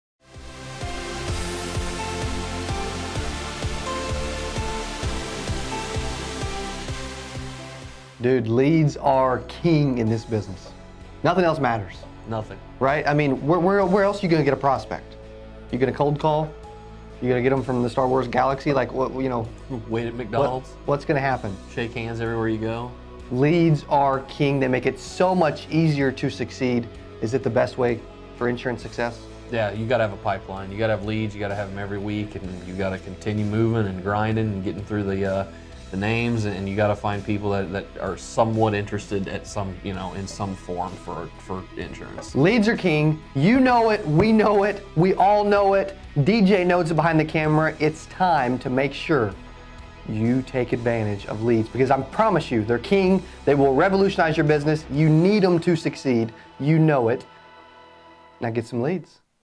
⚰️⚱️ Final Expense Lead Training - LEADS ARE KING!!! 👑
Learn how to work leads correctly and get more sales out of your leads.

Final Expense Lead Training with Rance & Cody

F.E.L.T.-EP4:  Cody and Rance are excited about "Real-Time" leads and how they are difference makers in the leads game.  If you want to have a shot at reaching 90% of your leads, Real-time is the way to go!!!